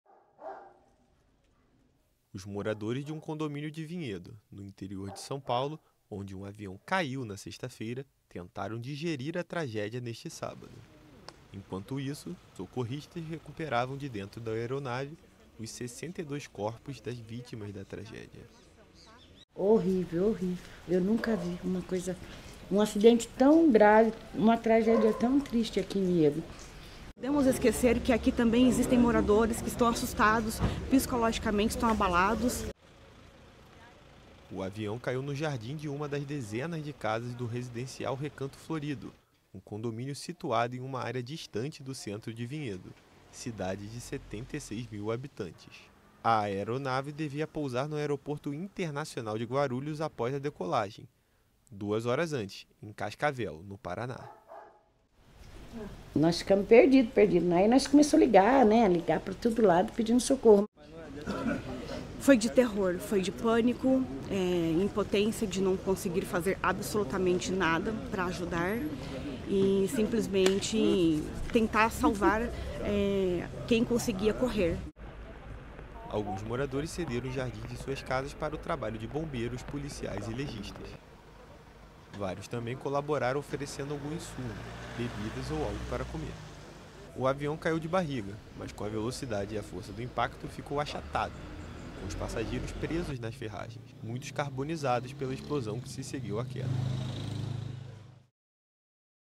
Moradores de Vinhedo narram momentos de terror após acidente aéreo | AFP
Moradores de Vinhedo contaram, neste sábado, os momentos de pânico vividos na véspera, quando um avião da companhia aérea Voepass caiu em um condomínio na cidade. Socorristas confirmaram que recuperar os 62 corpos das vítimas.